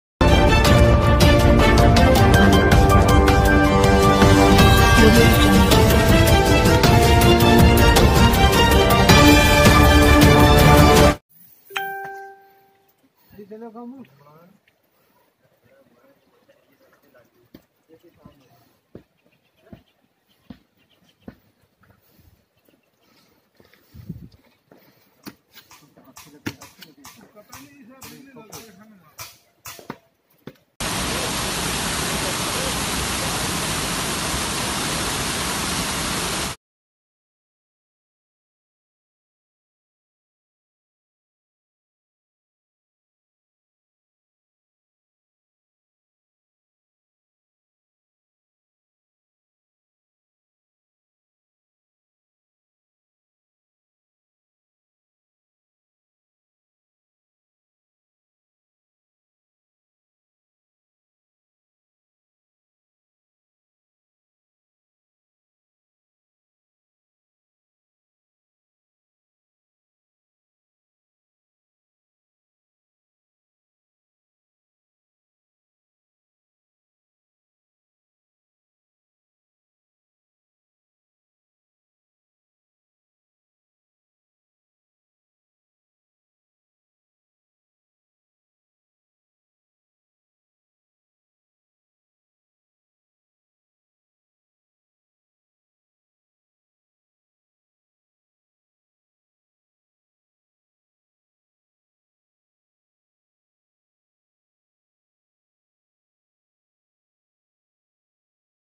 sanai link road nari chouhana blocked due to landslide plz subscribe our channel
Today sanai link road nari chouhana blocked due to landslide and locals of the area tryed to clear the slides and suffer alots